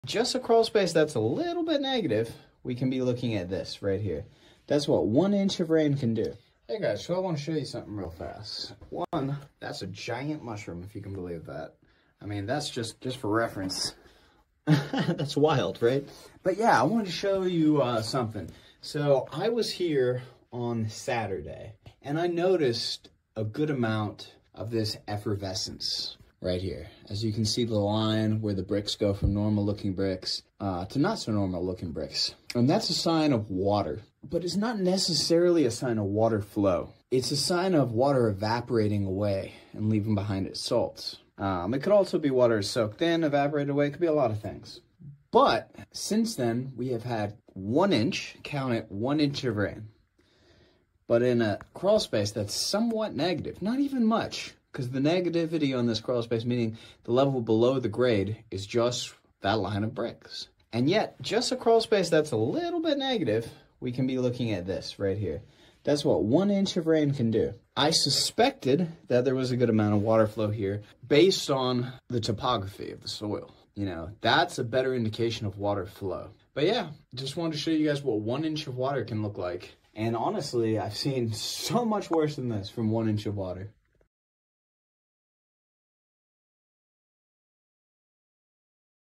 One Inch of Rain, Major Problems What Your Crawl Space Is Hiding
Want to know why I check topography before anything else? Because the soil tells the story.

Those white salt lines on the bricks? That's water evaporating and leaving evidence behind.

But the real proof came after just one inch of rain - suddenly we could see exactly where water flows when it rains.

This crawl space is barely below grade, just one line of bricks down, and it's still getting water intrusion.

I've seen way worse from the same amount of rain, but this is a perfect example of why even slight grade issues matter in the Lowcountry.